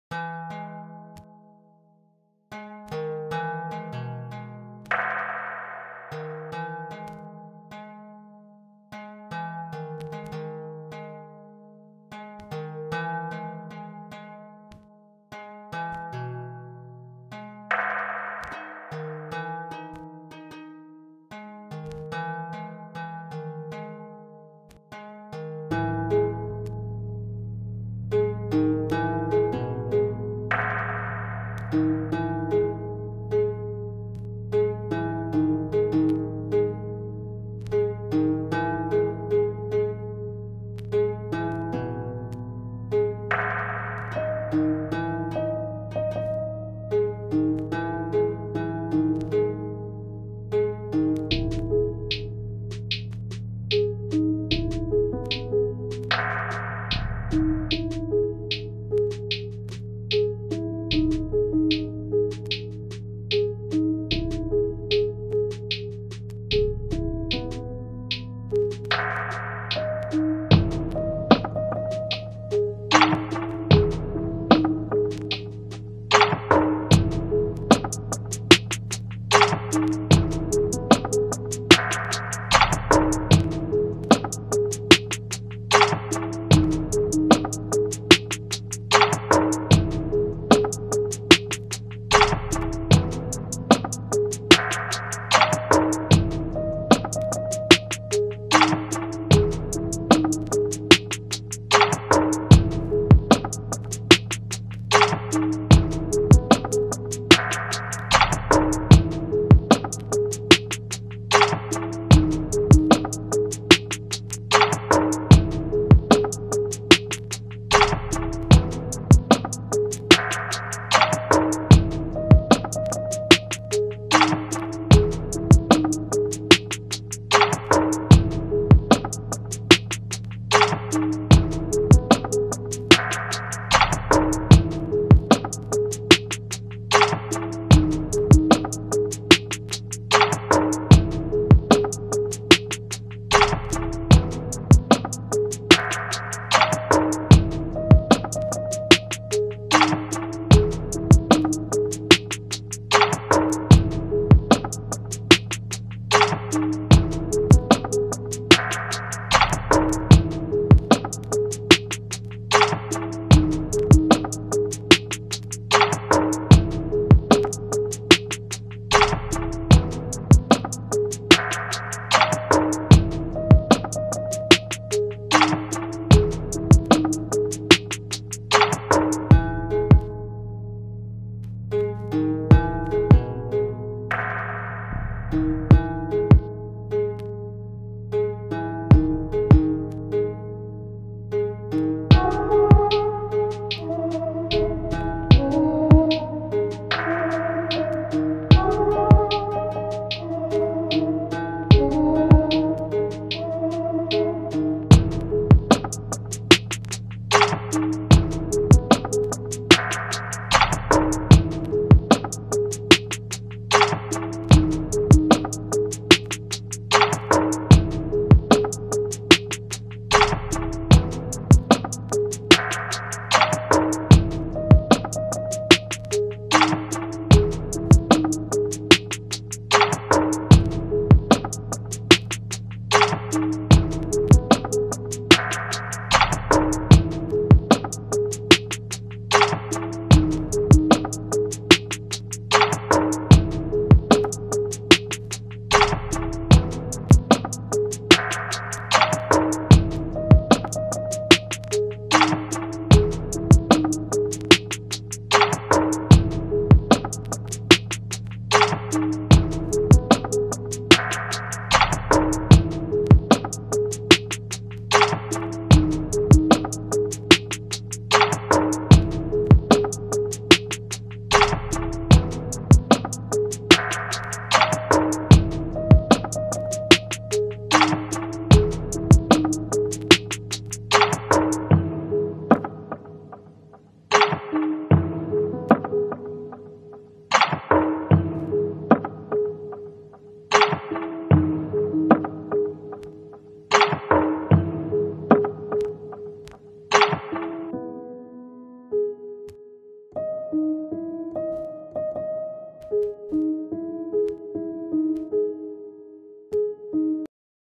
C l o u d P a l a c e | old-school LoFi Hip Hop to fly through space
Hello there, nice to have you here. Don't mind me, just trying out new animation stuff to accompany my music.

I don't know if this was worth the two hours of render time, but I like how it all reacts to the music playing^^

But besides the animation, I hope you like the music. While creating this I was thinking how it would feel just to fly above the clouds or even in space.

So sit back and fly with me :)